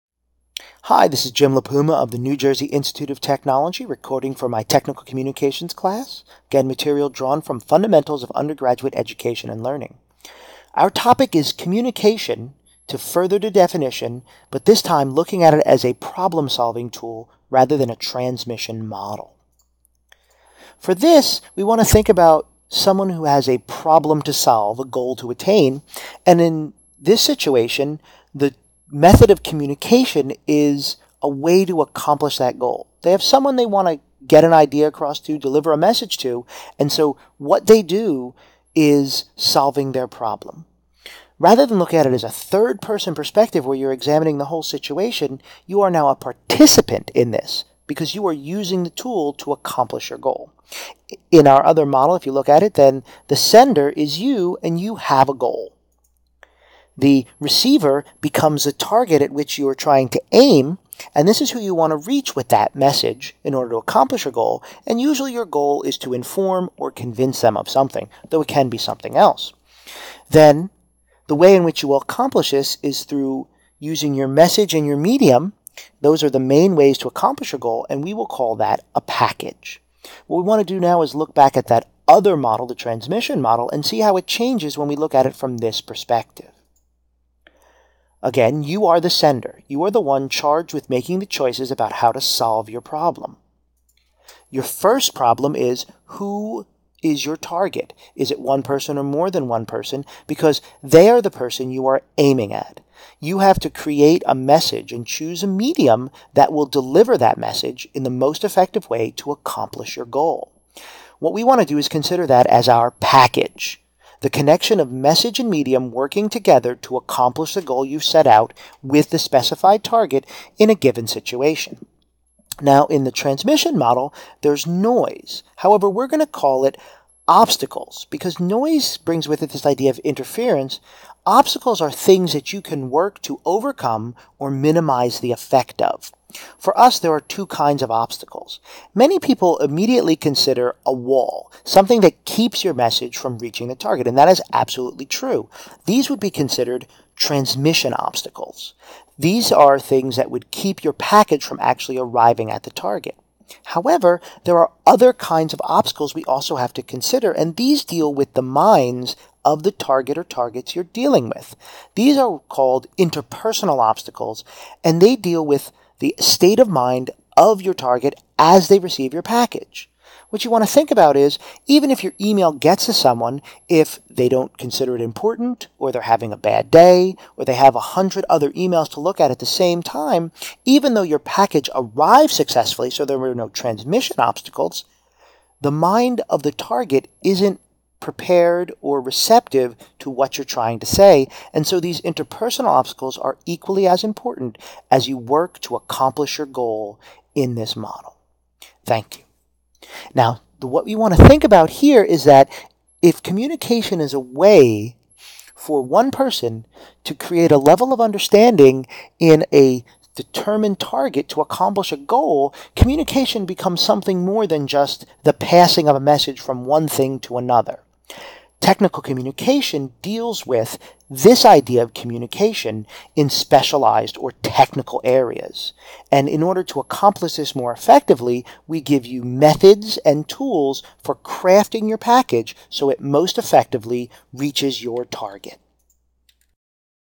ENG 352 - Technical Writing - 06 - Communication Pt 3 Problem Solving Tool
Lessons 04-07 move in sequence from a simple model of communication to a much more complex one that is dynamic and responsive.  This video presents the idea of communication as a tool to solve problems rather than the simple transfer model.  James Lipuma is the Instructor.